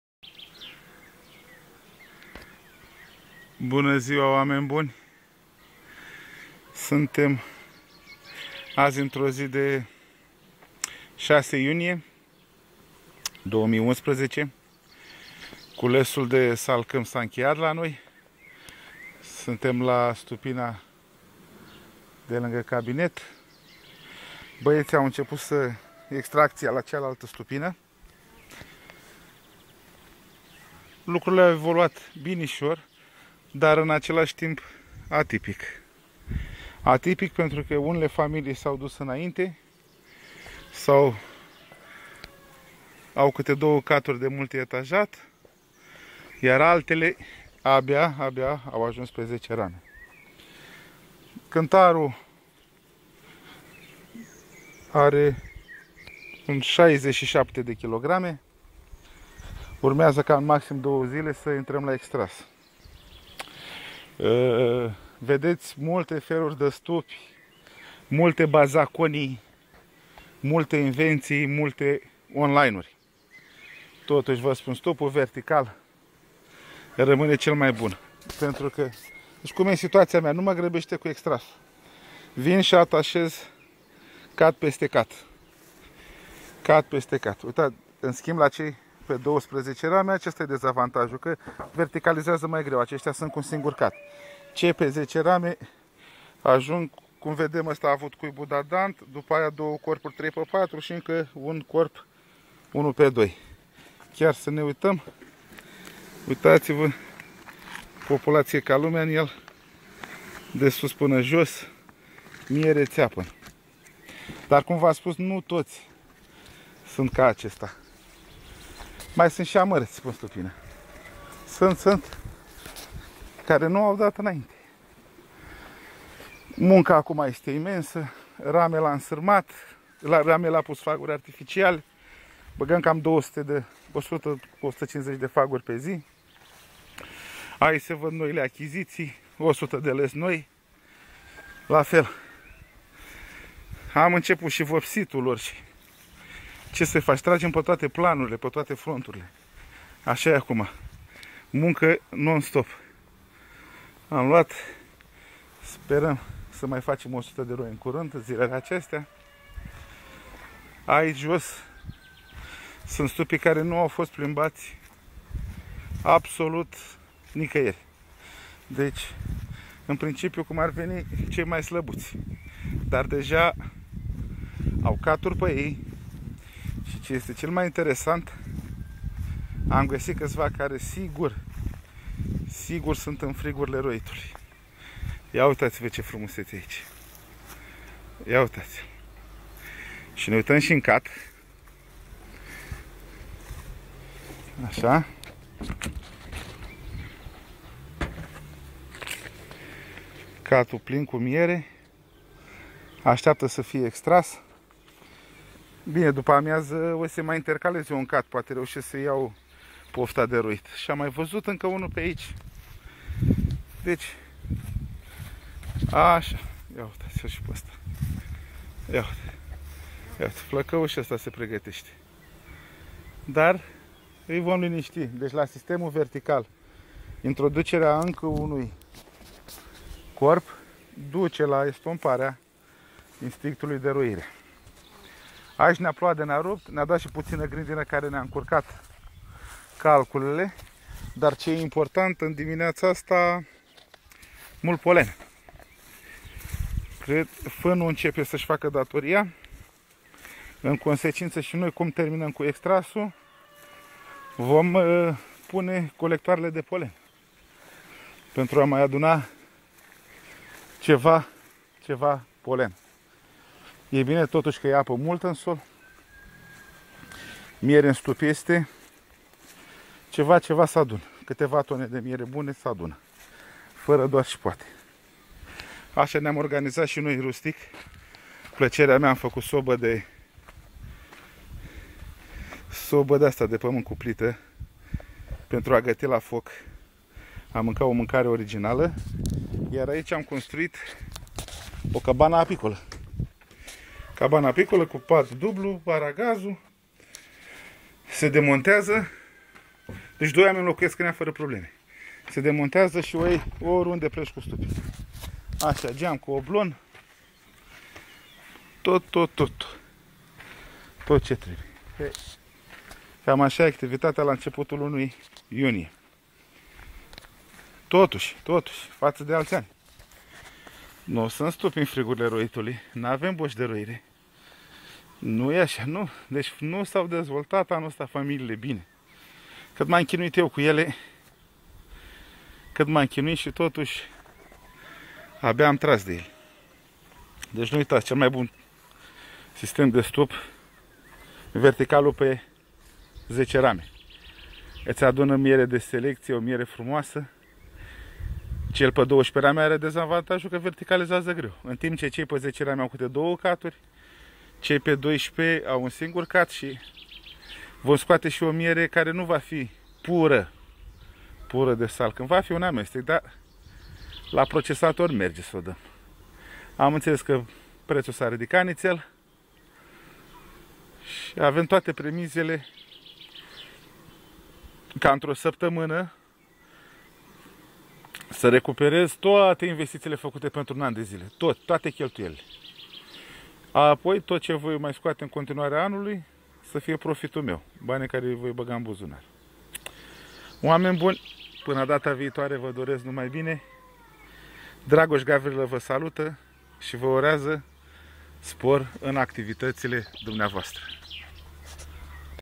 Salcam 2011 - primele concluzii
Anul 2011 a fost pana acum un an dificil dar totusi cu rezultate satisfacatoare.